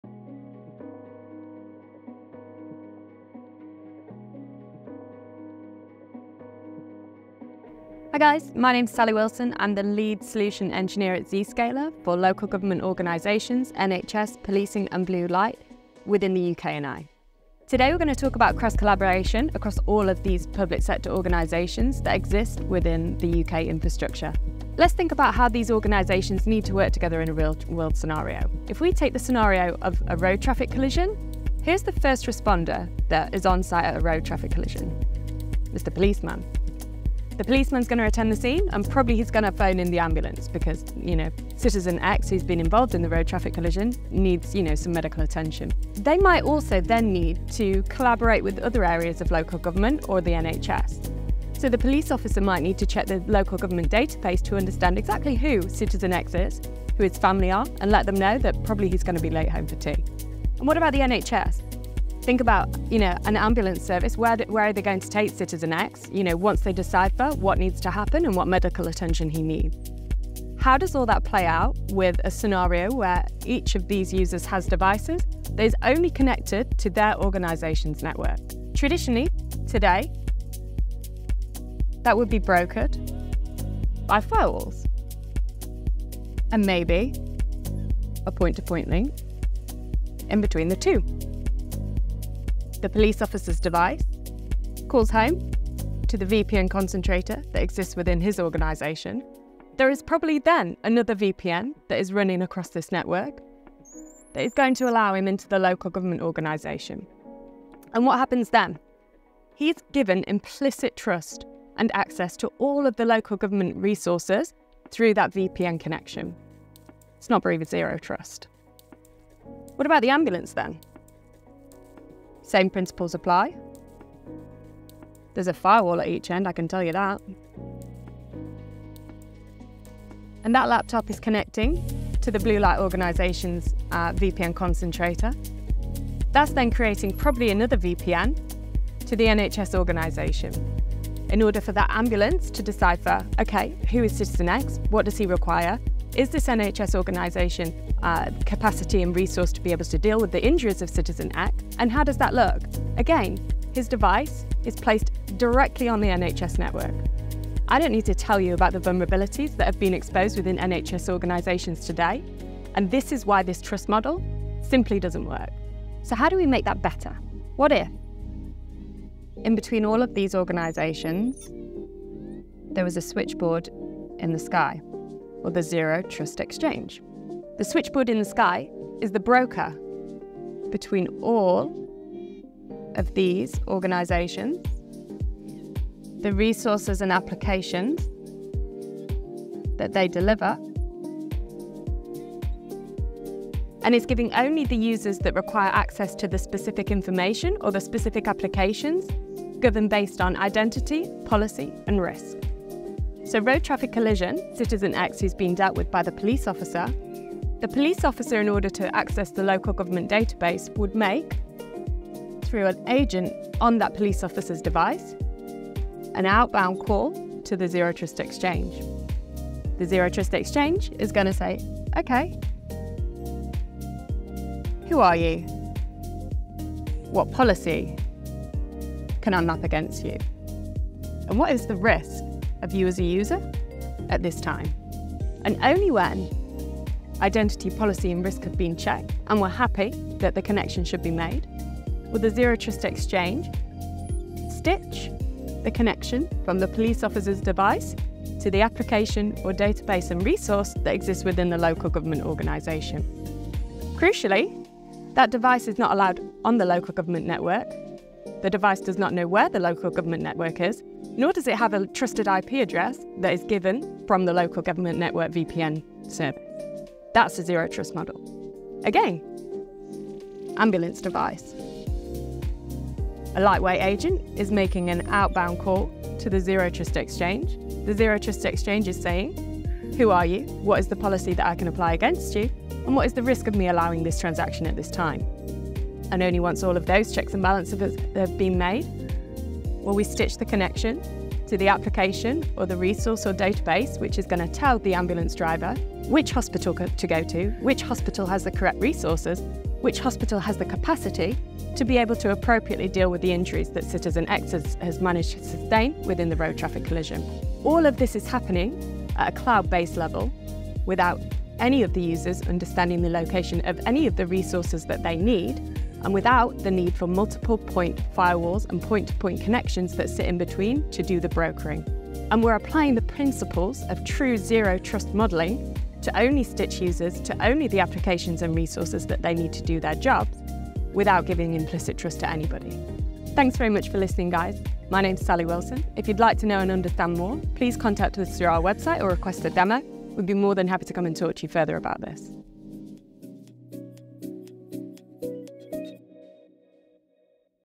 Zero Trust Networking for Public Sector
Zscaler enables the public sector to fully embrace the cloud and modernize IT by leveraging zero trust—securely connecting users to applications regardless of device, location, or network.
Our Zero Trust Exchange™ enables the seamless, secure exchange of information, transforming the way we work today and tomorrow.